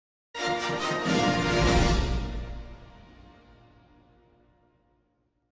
Fox Futaba Sakura Pictures Television logo (2019-) (Short #2 Version)
Credit to SuperPixar1467 for the model.

The short #2 version of 2019 Fox Futaba Sakura Pictures Television logo.

NOTE: Fake/Fan-made.